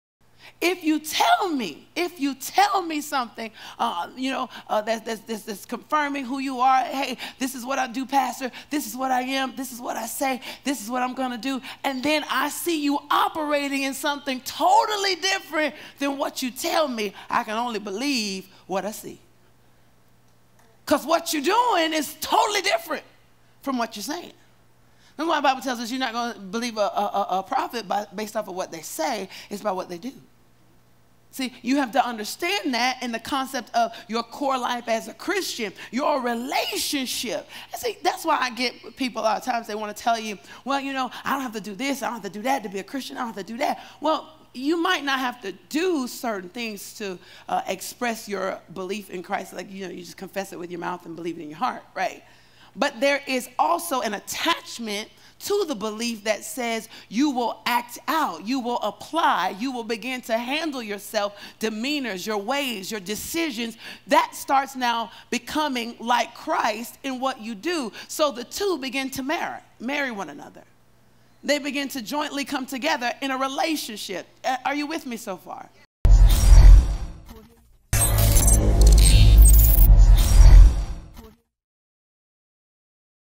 Pastor Richelle Lorde | Actions Speak Louder Than Words | Jesus People Ministries Miami
"Do not believe me unless I do the works of my Father. 38 But if I do them, even though you do not believe me, believe the works, that you may know and understand that the Father is in me, and I in the Father.”

- John 10:37–38

We’re committed to spreading the love of Christ throughout our Miami Gardens community and throughout the entire world.